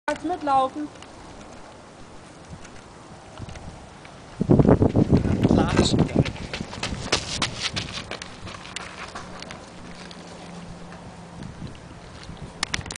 I against Me - Shooting mit Timo Bracht (Hawaii)
Hier seht Ihr Timo Bracht nach einer zügigen Radeinheit (2 Std.) hart koppeln. Extra für das Shooting wurde die Strecke um einen Hotspot gelegt, so dass wir mehrfach Material sammeln konnten.